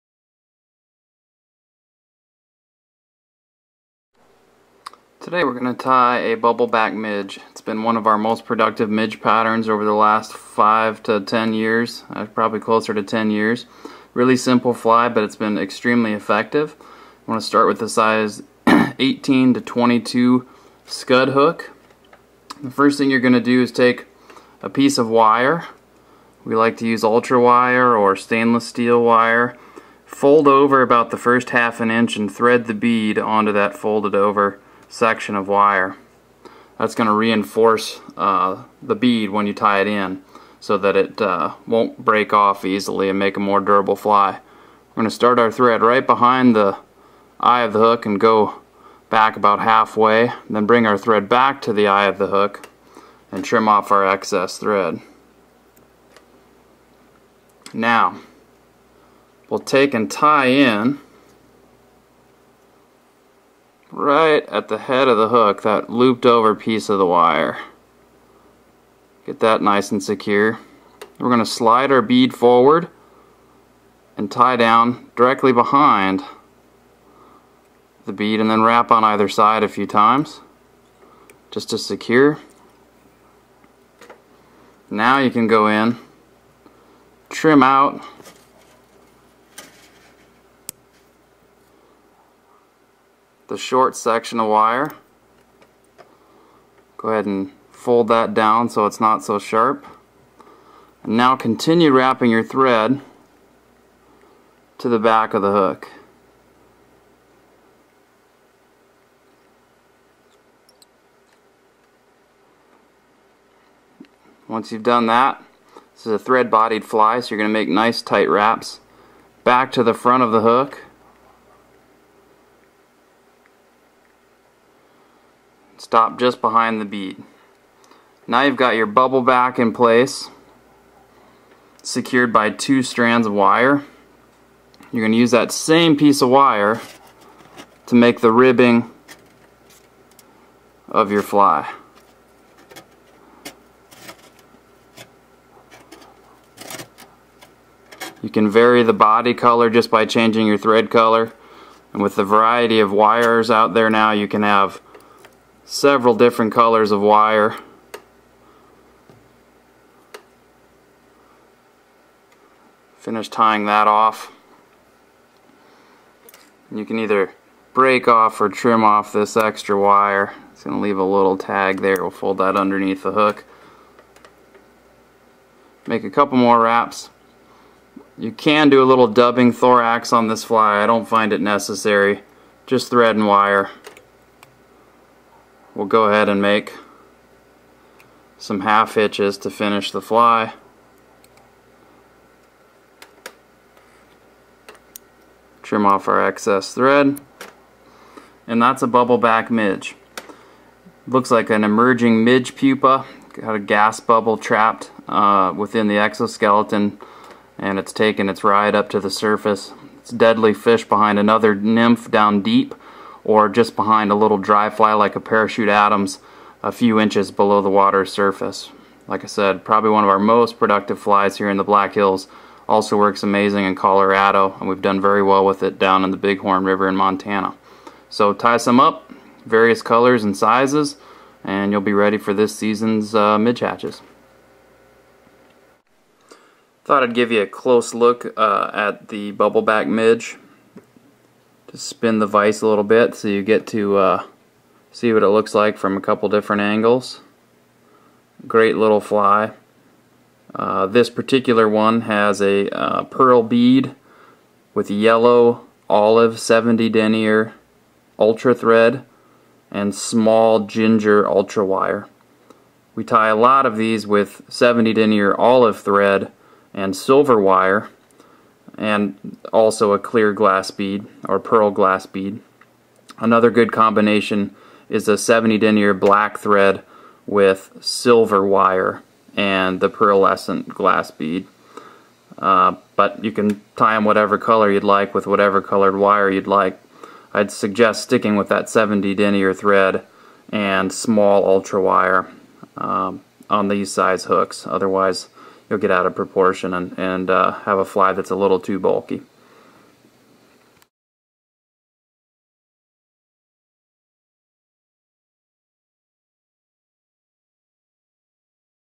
Fly Tying with Hans: Bubble Back Midge
Tying the bubble back midge. This fly gets the job done. Trail it behind another nymph or fish below a dry fly, it always works. The color scheme is up to you.

Tied by Hans Stephenson from Dakota Angler & Outfitter in Rapid City, South Dakota.